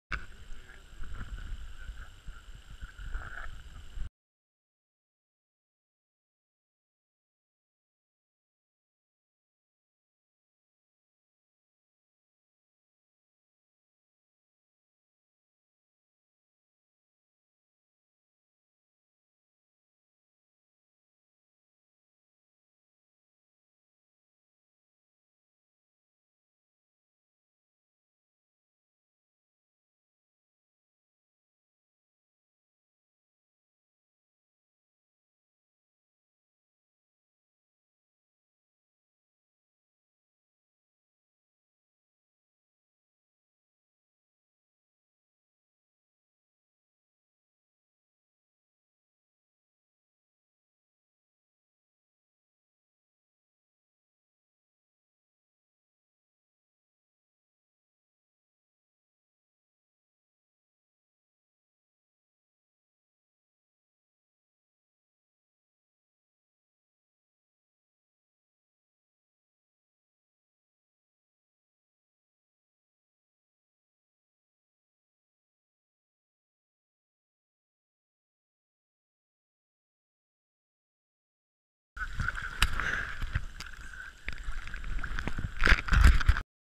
Pier Jumping in West Cork - Mizen Head, Ireland
Simple summer fun, pier jumping at Toor Pier, West Cork, very far off the beaten path in those parts!